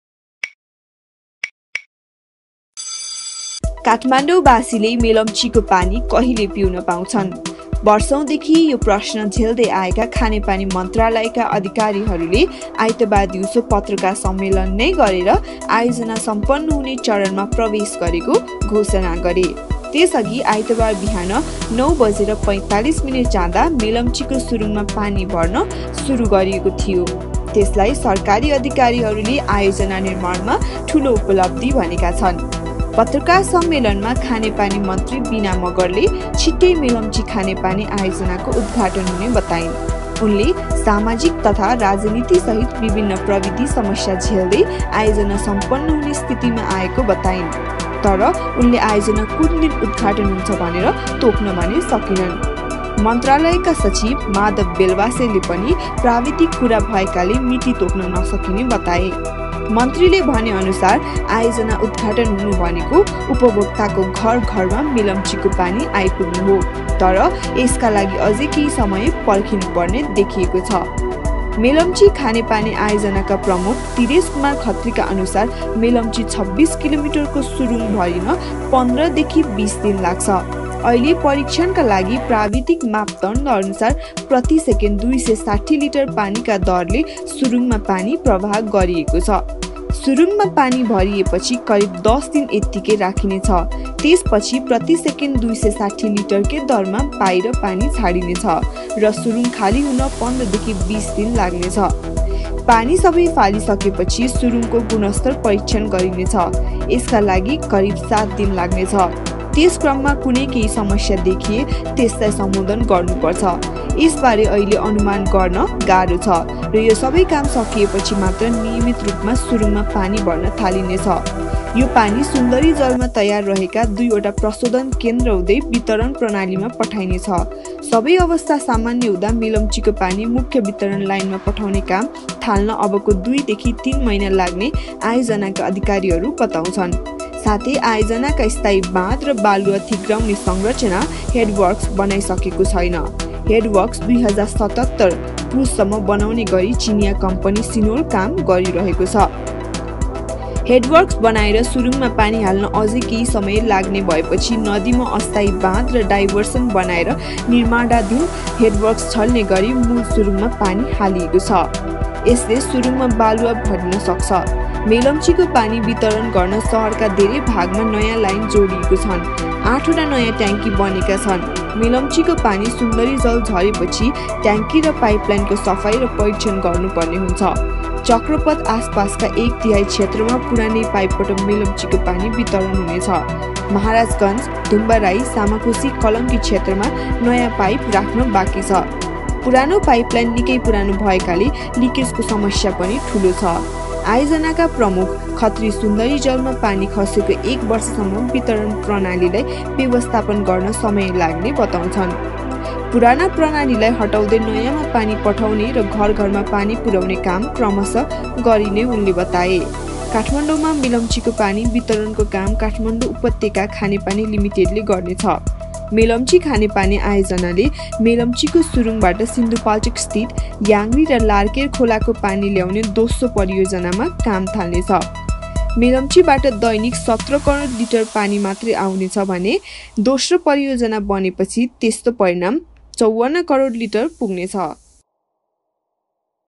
अब छिट्टै धाराधारमा आउनेभयो मेलम्चीको पानी।काठमाण्ड़ुवासीहरुको लागि आयो खुसीको खबर।
Hamro Gaughar